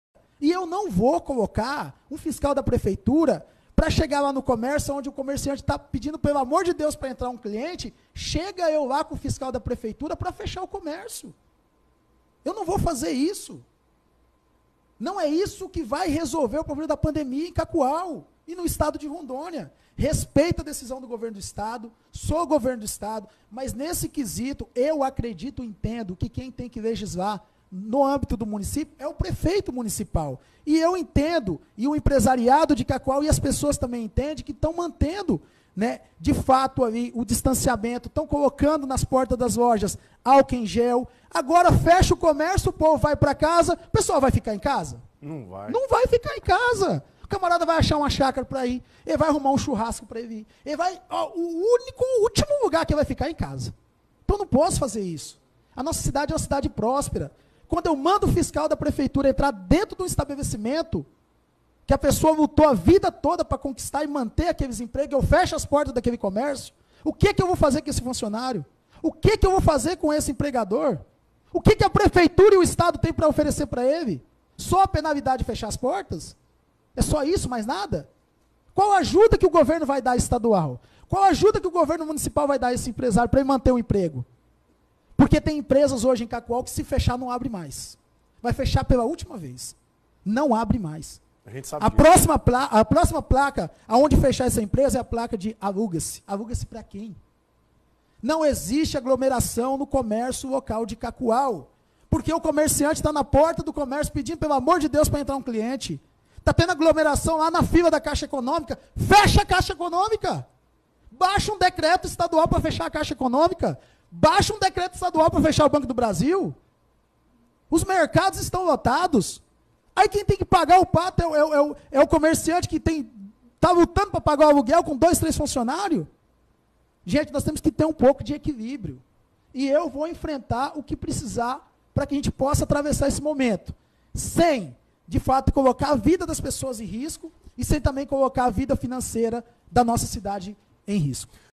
Veja o exemplo do Prefeito de Cacoal!
Parabéns Prefeito de Cacoal!. Ainda existe homens de verdade nesse país, com caráter, respeito e honestidade. Existem pessoas honesta na política. Quando entra alguém desonesto na política é pq vc colocou ele lá e a culpa é de quem? E muitos q colocam vagabundo no poder é pq compactua com o errado, se vende, vende voto. Se vc se indentifica com corruptos, vagabundos, ladrões, vc não é diferente deles.